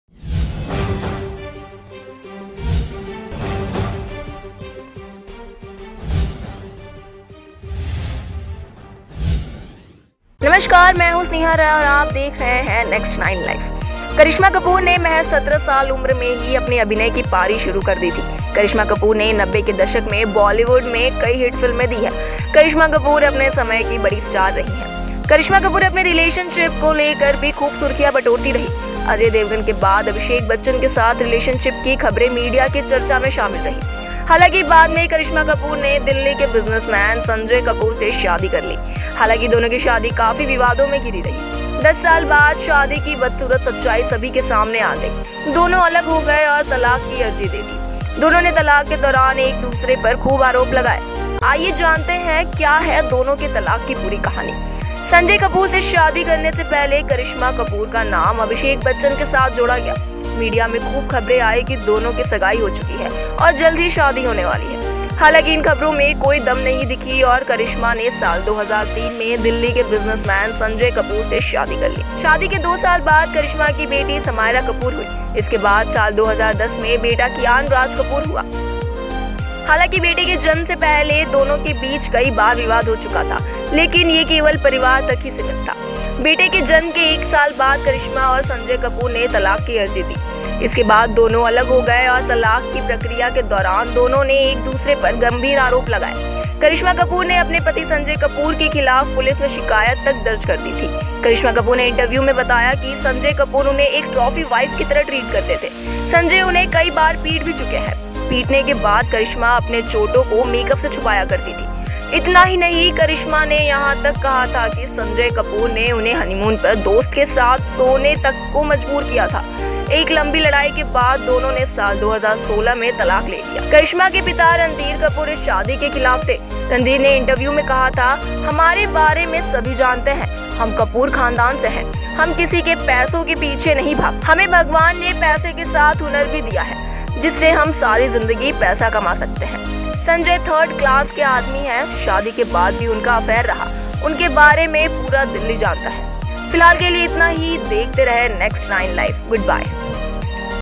पिता के वजह से हुआ करिश्मा कपूर का तलाक, 13 साल बाद हुआ बाड़ा खुलासा….
karishma kapoor